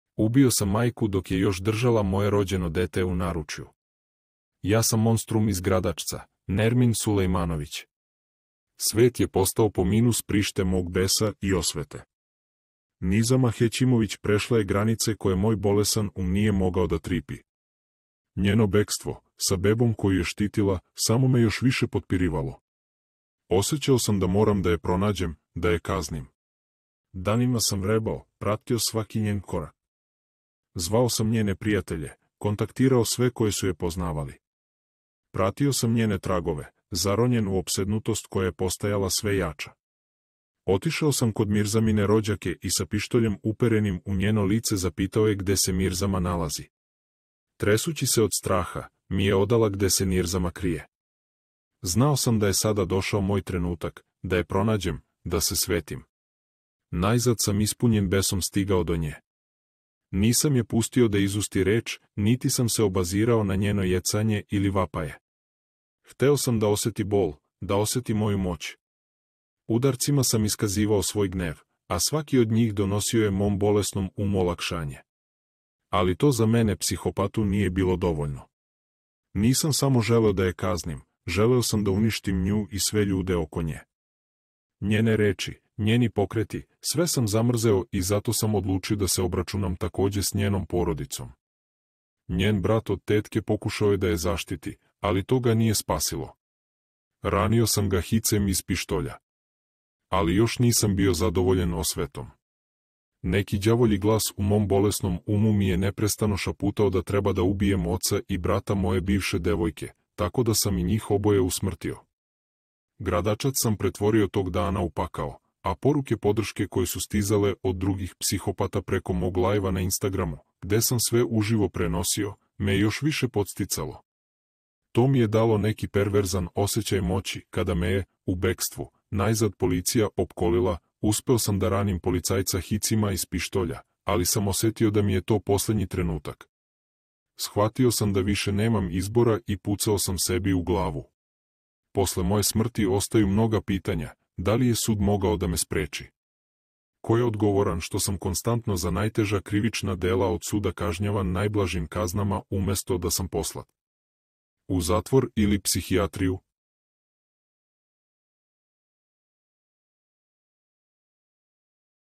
Monstrum iz Gradačca ili zašto je bodibilder Nermin Sulejmanović učinio krvopoliće?
ko je bio bodibilder Nermin Sulejmanović iz bosanskog Gradačca,koji je ubio ženu,još 3 ljudi i ranio 4?